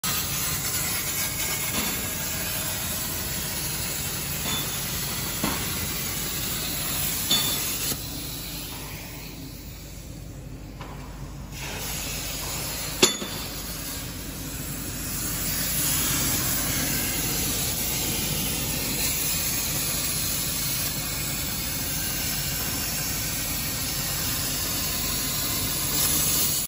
cnc plasma cutting machine cut metal sheet videos in customer factory
CNC plasma cutting machine specialize in cutting metal sheet materials, this videos take in our one customer's factory, producing some metal parts for industrial machine.
if interested, welcome to inquiry details